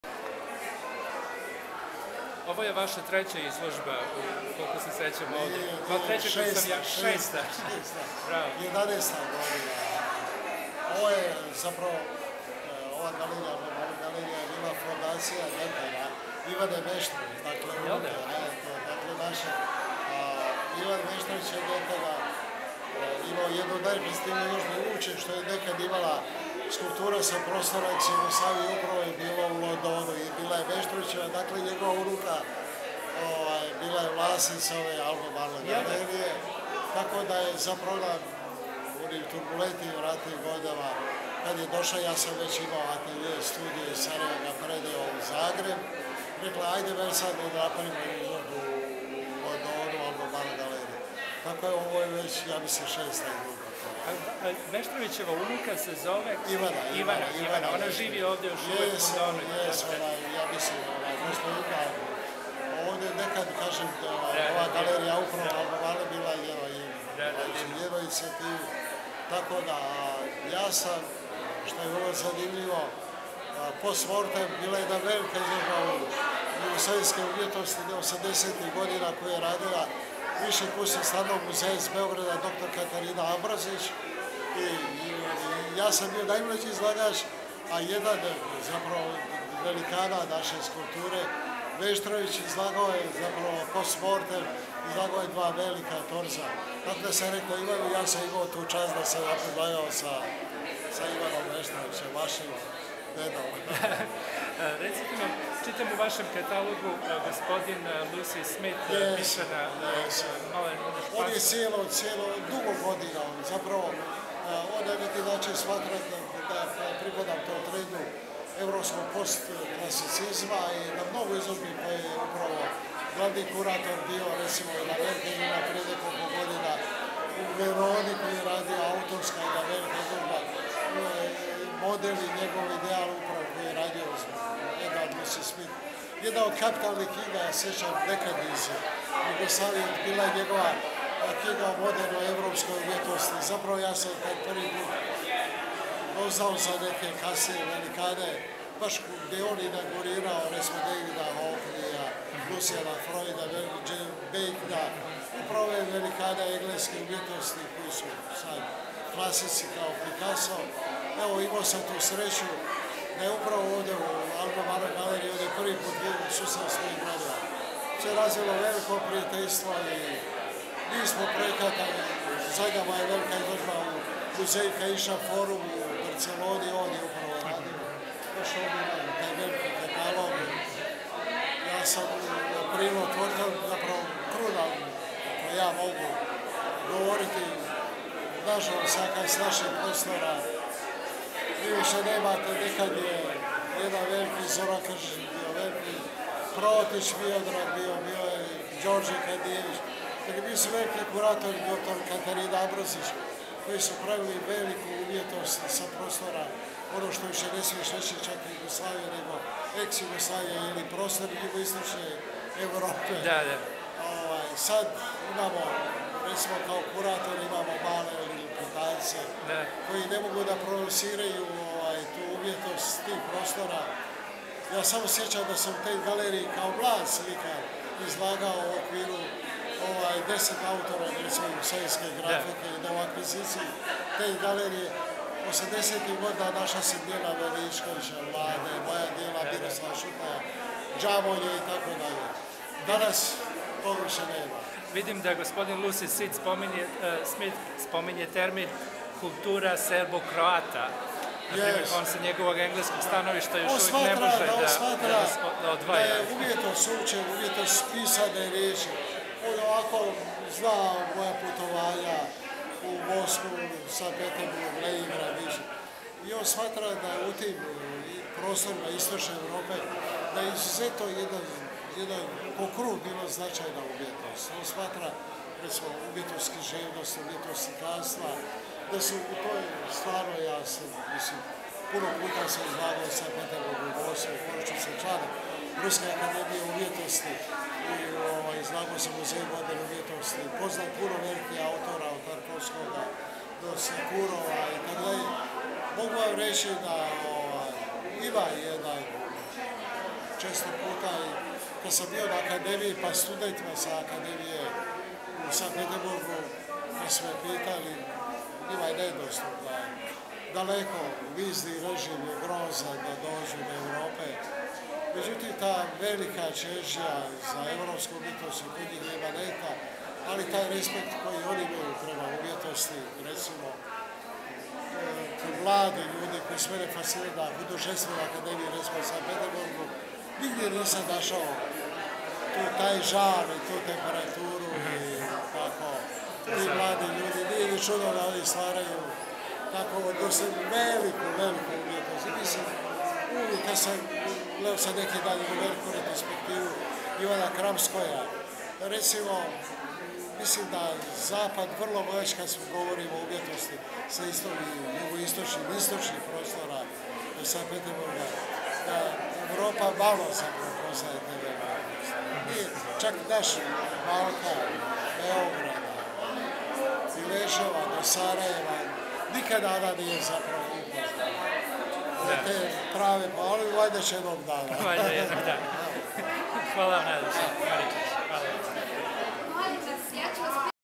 Mersad Berber o slikarstvu 21 vjeka
Mersad Berber at the Albermarle Gallery in 2009, interviewed by Vladimir Maricic